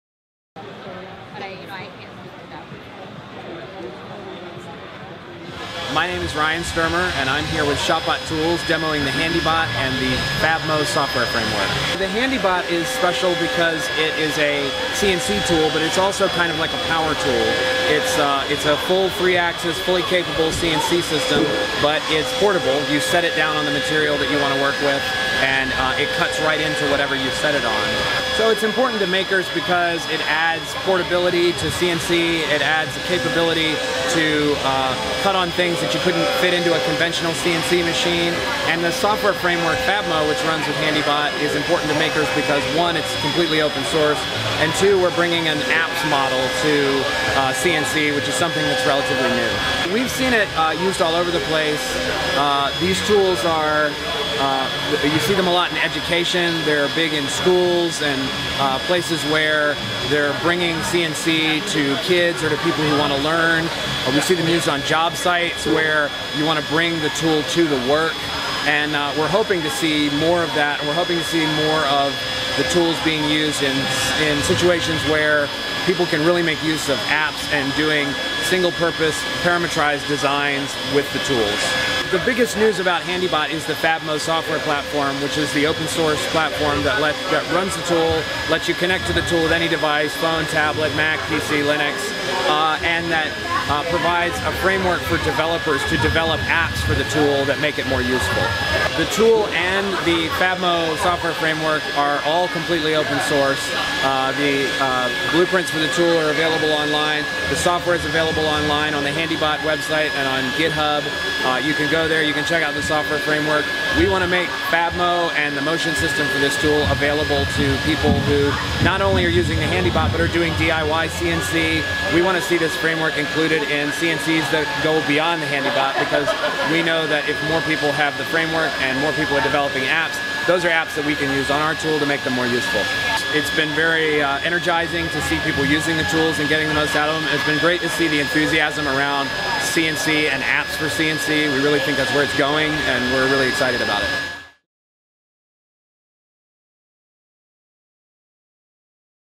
Watch Handibot Cut 3D Chess Pieces
Shopbot brought Handibot to Maker Faire to show off the new FabMo Software.